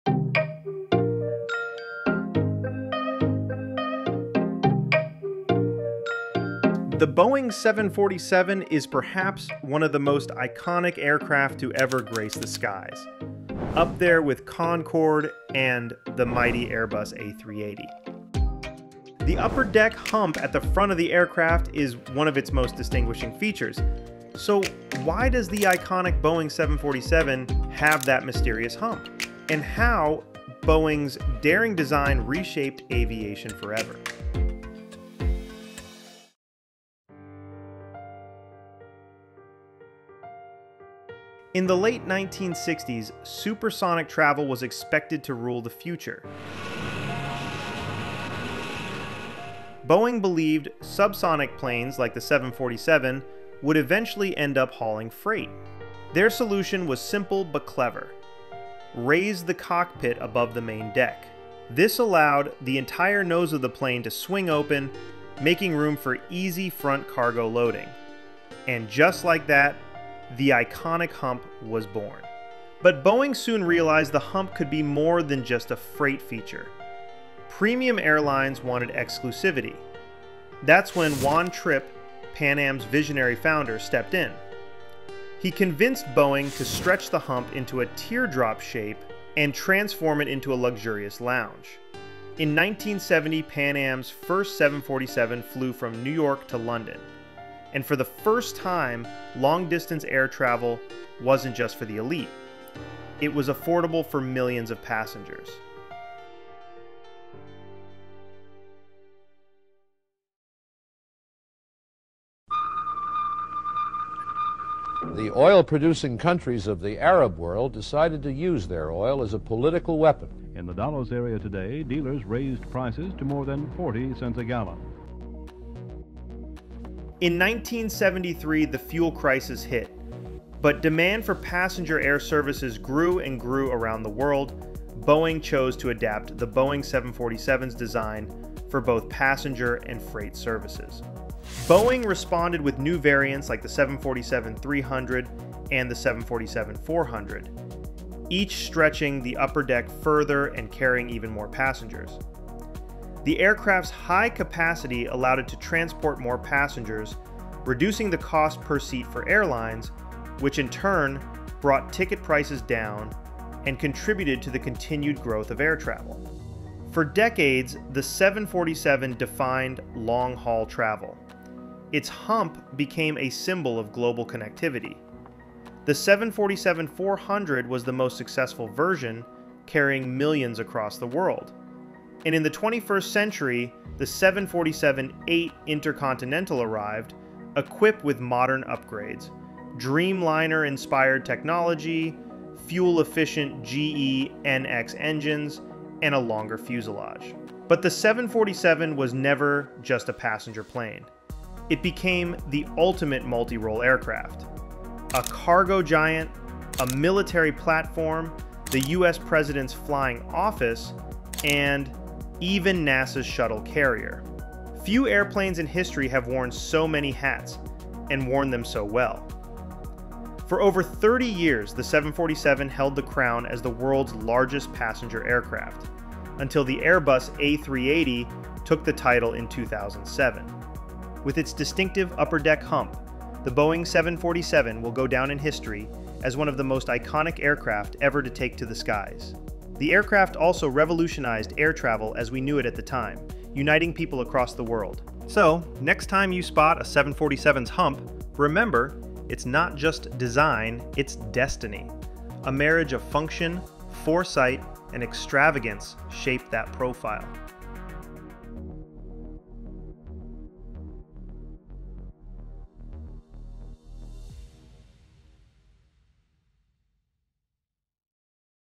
The Secret Behind the Boeing 747’s Iconic Hump
Ever wondered why the Boeing 747 — the Queen of the Skies — has that strange hump at the front?
It wasn’t built for looks. In fact, Boeing never designed it for passengers at all.

In this video, we uncover the fascinating story of how the hump was born for cargo, transformed into a luxury lounge in the sky, and became one of the most iconic designs in aviation history. From Pan Am’s first 747 flight, to the aircraft’s role as Air Force One and even NASA’s Space Shuttle carrier — the story of the hump is a story of aviation itself.

✈️ Discover how a simple engineering decision reshaped global travel forever.